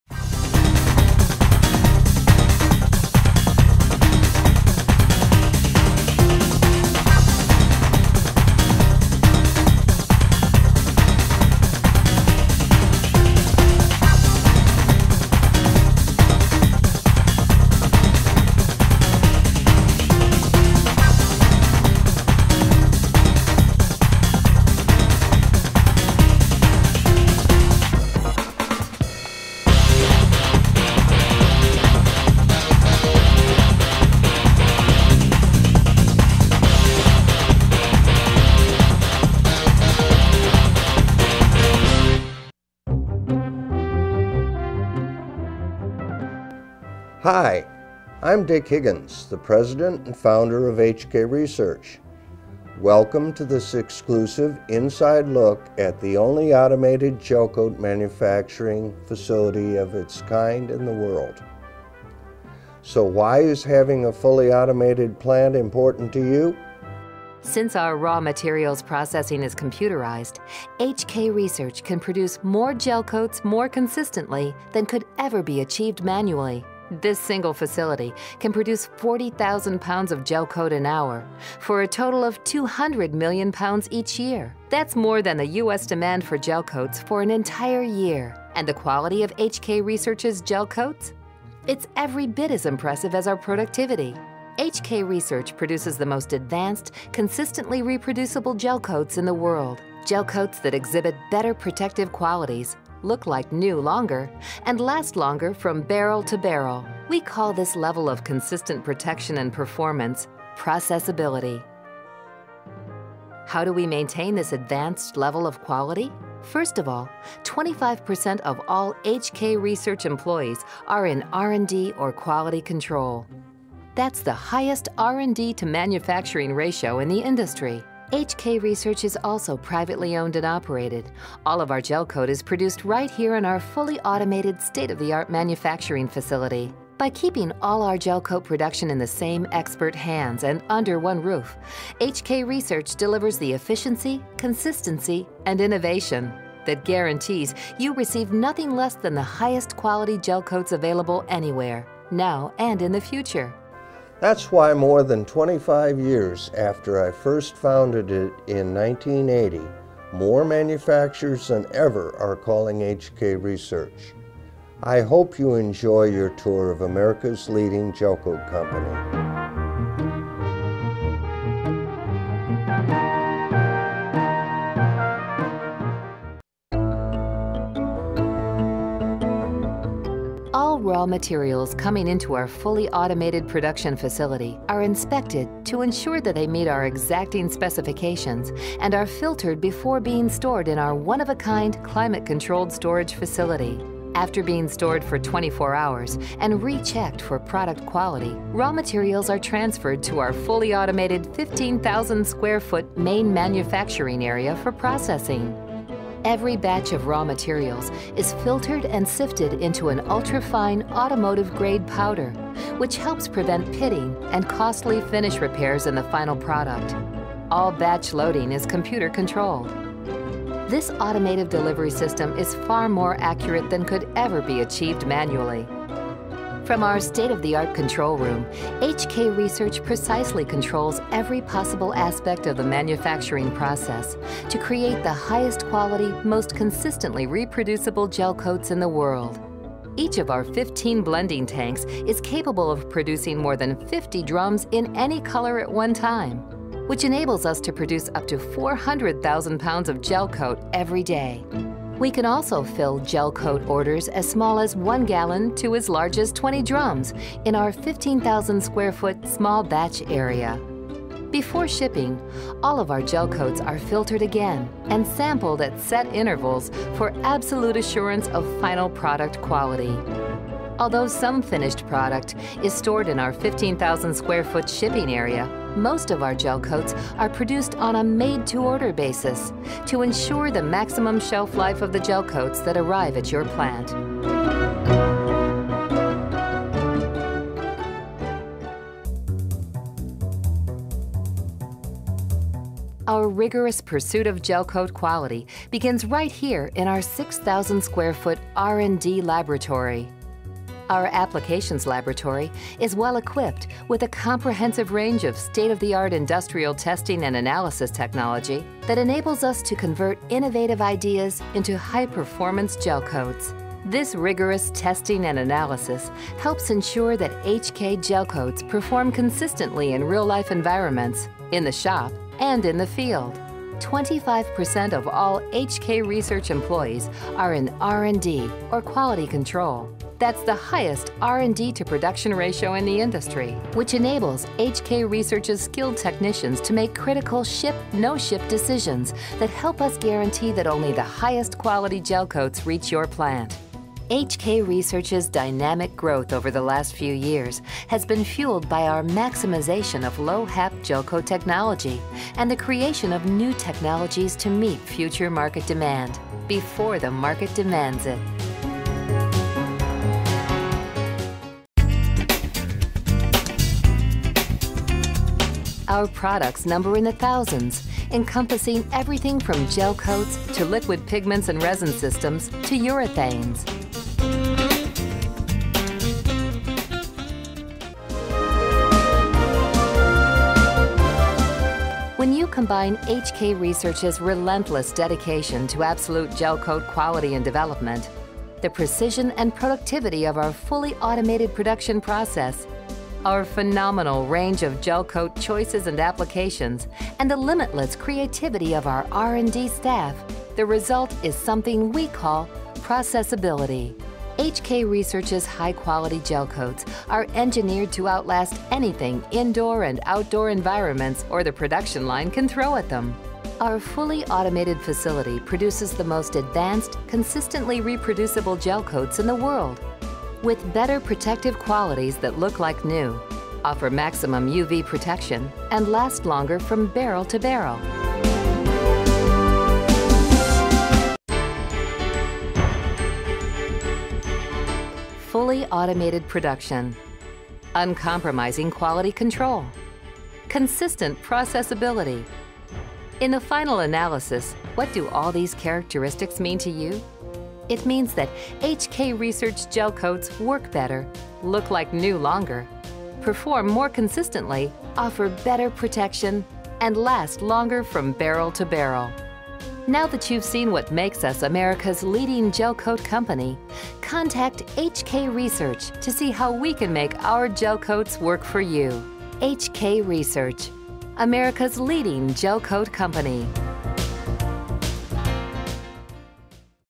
HK Research Plant Tour
Fully automated manufacturing facility designed specifically for the manufacture of polyester gel coats!